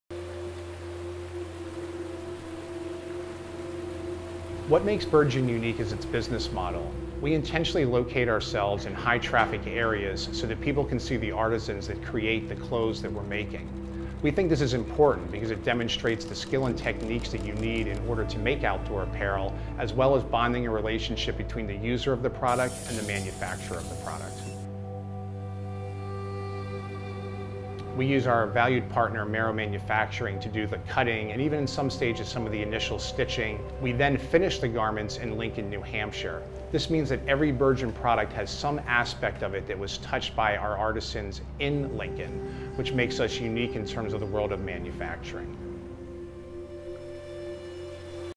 Burgeon's Mission
Burgeon Founder Rudy Glocker speaks to the mission of Burgeon to positively impact mountain communities